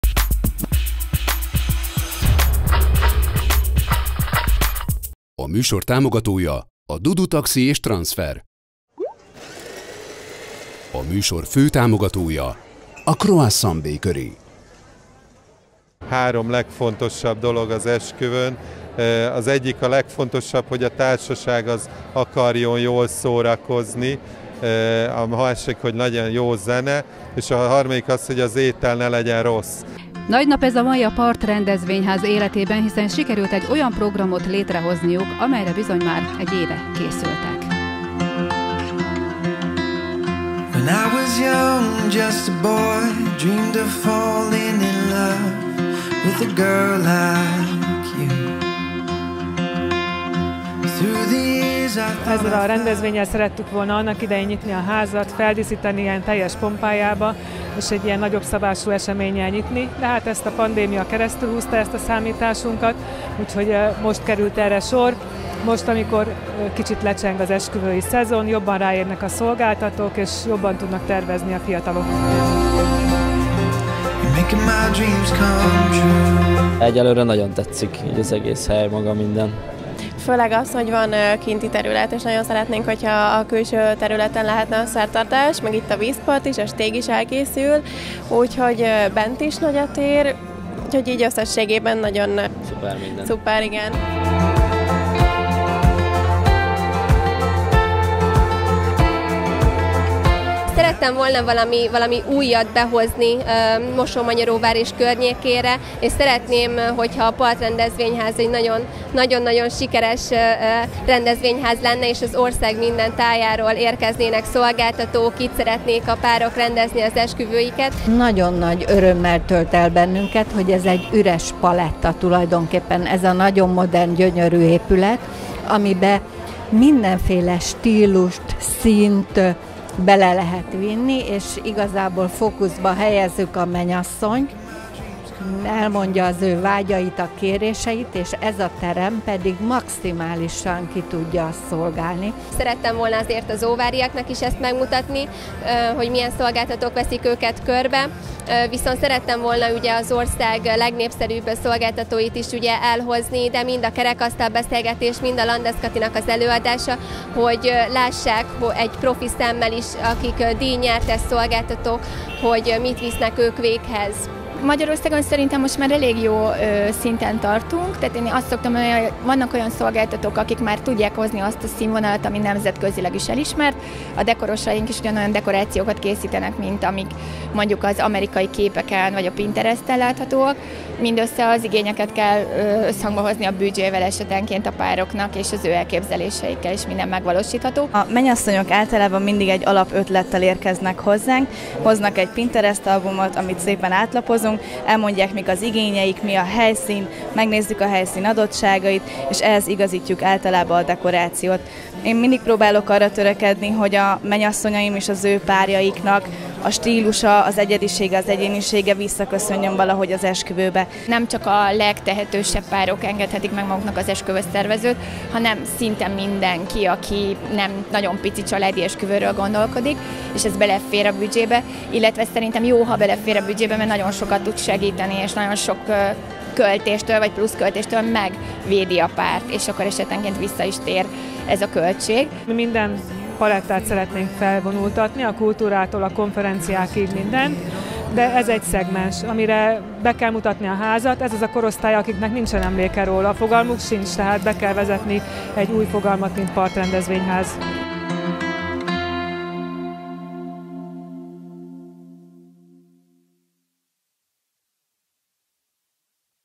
A Nagy Nap 2021.
És bár már egy évvel korábban szerették volna, a világban zajló jól ismert események miatt idén októberben tudták a PART Rendezvényház NAGY NAP c. eseményét megszervezni. Érdemes volt rá várni, hiszen aki ellátogatott, megtapasztalhatta, mennyi minden is fontos ahhoz, hogy egy esküvő valóban a legjobb legyen - és hogy a célra milyen kiváló helyszínt biztosít az intézmény. Szemnek, szájnak, léleknek való kínálatot biztosítottak szombaton mindenkinek a szervezők.

SZTV 2021®